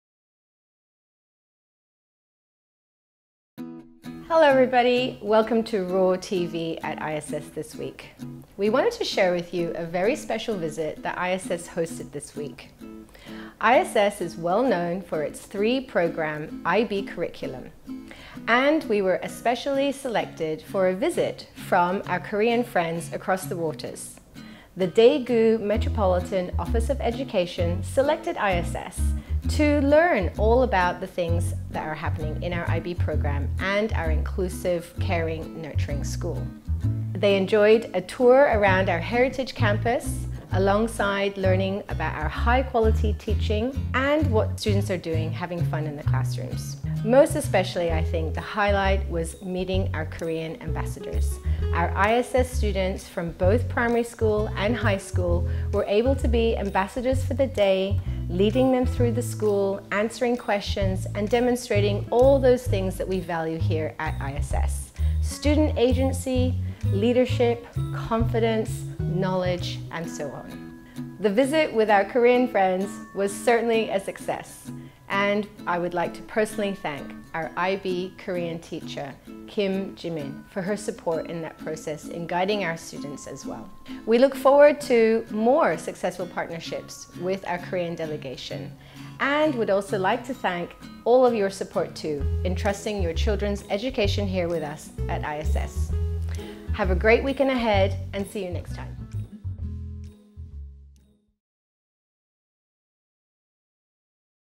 ISS International School I Daegu Metropolitan Office of Education
This week, we welcomed a delegation from the Daegu Metropolitan Office of Education. Miss Fiona Edwards (Academic Director of Pastoral and Head of EAL), together with our Korean student ambassadors, and Miss Kim Jimin (MYP Korean Language & Literature, and DP Korean A Teacher), gave them a tour of ISS and shared valuable resources regarding the IB curriculum.